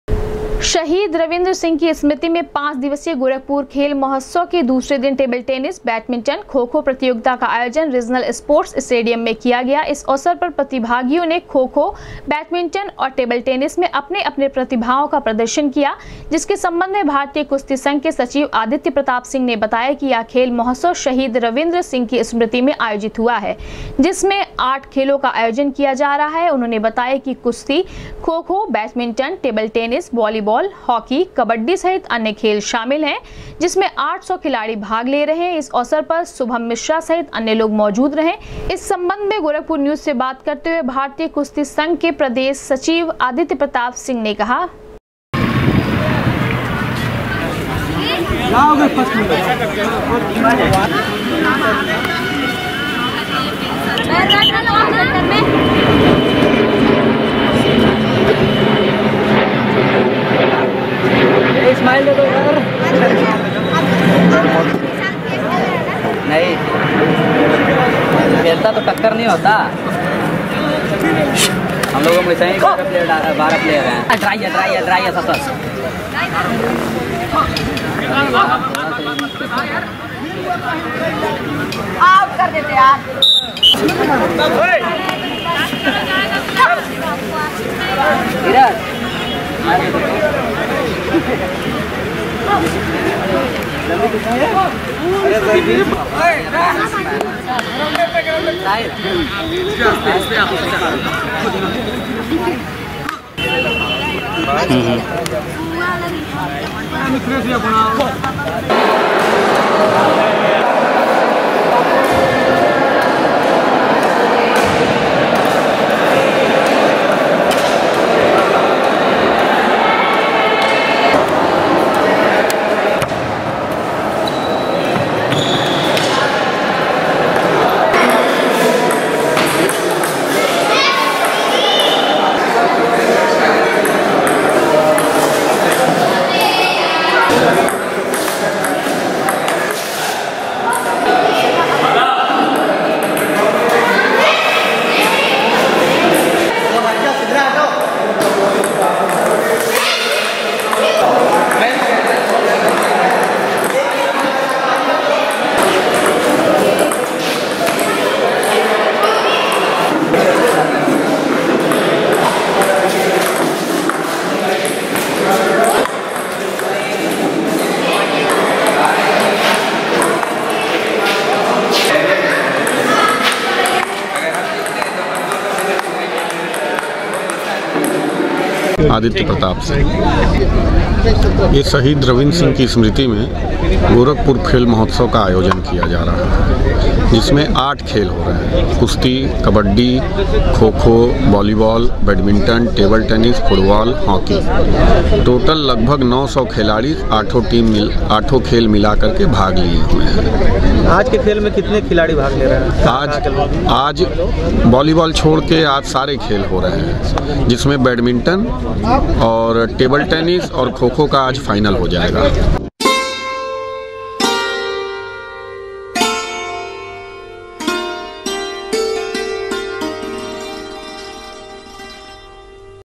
शहीद रविंद्र सिंह की स्मृति में आयोजित खेल महोत्सव के दूसरे दिन टेबल टेनिस ,बैडमिंटन, खो- खो
शहीद रविंद्र सिंह की स्मृति में आयोजित खेल महोत्सव के दूसरे दिन टेबल टेनिस ,बैडमिंटन, खो- खो प्रतियोगिता का हुआ आयोजन, प्रतिभागियों ने किया अपनी-अपनी प्रतिभा का प्रदर्शन